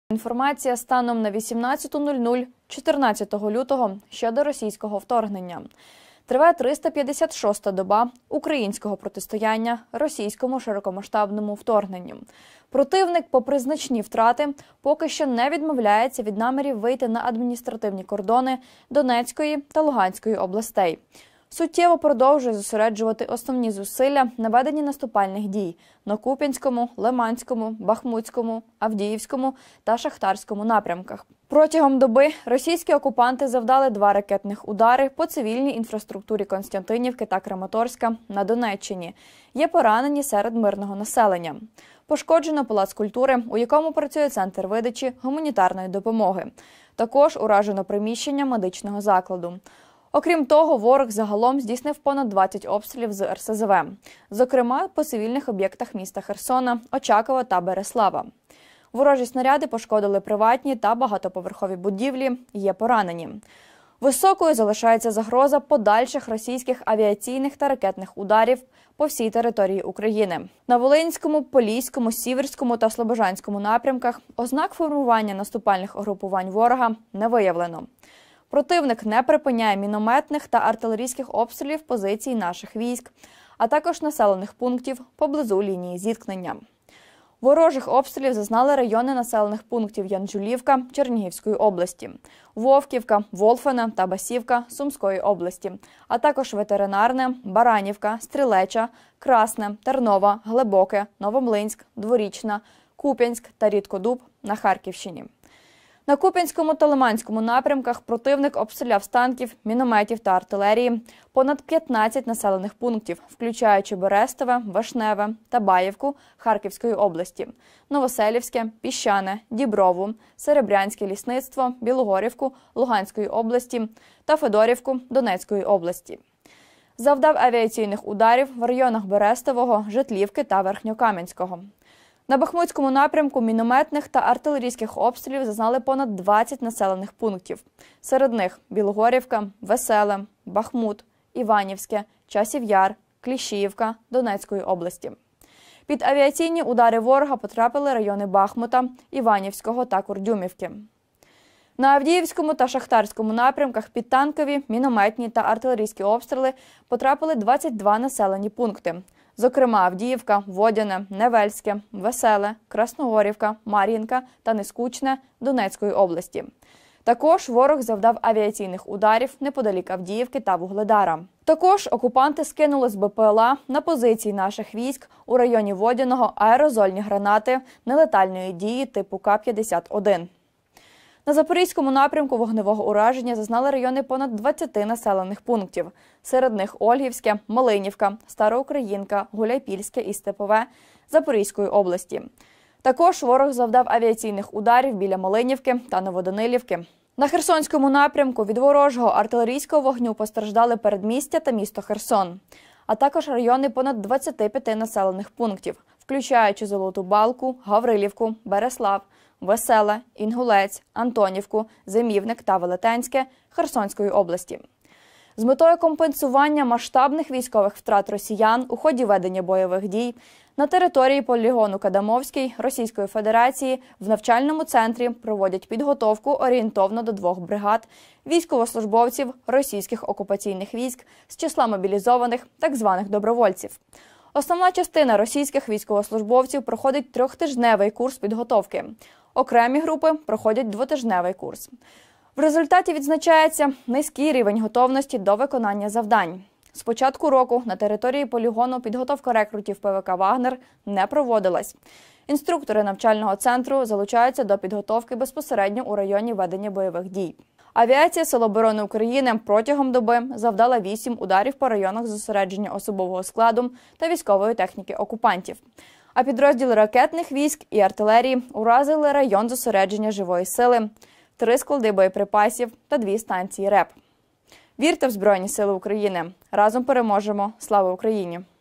Оперативна інформація станом на 18:00 14.02.2023 щодо російського вторгнення
Слава Україні! Триває триста п’ятдесят шоста доба російського широкомасштабного вторгнення.
Противник, попри значні втрати, поки що не відмовляється від намірів вийти на адміністративні кордони Донецької та Луганської областей.  Суттєво продовжує зосереджувати основні зусилля на веденні наступальних дій на Куп’янському, Лиманському, Бахмутському, Авдіївському та Шахтарському напрямках. Активно діє ворожа авіація.
Протягом доби російські окупанти завдали 2 ракетних удари по цивільній інфраструктурі Костянтинівки та Краматорська на Донеччині. Є поранені серед мирного населення. Пошкоджено палац культури, в якому працює центр видачі гуманітарної допомоги. Також уражено приміщення медичного закладу.
Окрім того, ворог загалом здійснив понад 25 обстрілів з реактивних систем залпового вогню, зокрема, по цивільних об’єктах Херсона, Очакова та Берислава. Ворожі снаряди пошкодили приватні та багатоповерхові будинки, є поранені.
Високою залишається загроза подальших російських авіаційних та ракетних ударів по всій території України.
На Куп’янському та Лиманському напрямках противник обстріляв з танків, мінометів та артилерії понад 15 населених пунктів, включаючи   Берестове, Вишневе і Табаївку Харківської області; Новоселівське, Стельмахівку, Невське, Червонопопівку, Піщане, Діброву, Серебрянське лісництво, Білогорівку Луганської області та Федорівку - Донецької. Завдав авіаційних ударів в районах Берестового, Житлівки та Верхньокамянського.
На Бахмутському напрямку мінометних та артилерійських обстрілів зазнали понад 20 населених пунктів, серед них – Білогорівка, Веселе, Парасковіївка, Залізнянське, Бахмут, Іванівське, Часів Яр, Кліщіївка, Залізне, Нью-Йорк Донецької області. Під авіаційні удари ворога потрапили райони Бахмута, Іванівського та Курдюмівки.
На Авдіївському та Шахтарському напрямках під танкові, мінометні та артилерійські обстріли потрапили 22 населені пункти, зокрема, Авдіївка, Водяне, Невельське, Веселе, Красногорівка, Мар’їнка, Георгіївка, Вугледар, Велика Новосілка, Времівка та Нескучне Донецької області. Також ворог завдав авіаційних ударів неподалік Авдіївки та Вугледара. Також, окупанти скинули з БпЛА на позиції наших військ у районі Водяного аерозольні гранати нелетальної дії типу К-51.
На Запорізькому напрямку вогневого ураження зазнали райони понад 20 населених пунктів.  Серед них - Ольгівське, Малинівка, Староукраїнка, Гуляйпільське, Мала Токмачка, Оріхів, Новоандріївка і Степове Запорізької області. Також ворог завдав авіаційних ударів біля Малинівки та Новоданилівки.
На Херсонському напрямку від ворожого артилерійського вогню постраждали передмістя та місто Херсон, а також райони понад 25 населених пунктів, включаючи Золоту Балку, Гаврилівку, Берислав, Веселе, Інгулець, Антонівку, Зимівник, Білозерку, Дніпровське та Велетенське Херсонської області.
З метою компенсування масштабних військових втрат росіян у ході ведення бойових дій, на території полігону Кадамовський російської федерації, в навчальному центрі проходять підготовку орієнтовно до двох бригад військовослужбовців російських окупаційних військ, з числа мобілізованих та "добровольців". Основна частина російських військовослужбовців проходить тритижневий курс підготовки. Окремі групи проходять двотижневий курс.
В результаті, відзначається низький рівень готовності до виконання завдань. З початку цього року, на території полігону підготовка рекрутів ПВК “вагнер” не проводилась. Інструктори навчального центру залучаються до підготовки безпосередньо у районі ведення бойових дій.
Авіація Сил оборони протягом доби завдала 8 ударів по районах зосередження особового складу та військової техніки окупантів, а підрозділи ракетних військ і артилерії уразили район зосередження живої сили, 3 склади боєприпасів та 2 станції РЕБ ворога.
Вірте у Збройні Сили! Наближаємо перемогу разом!
Слава Україні!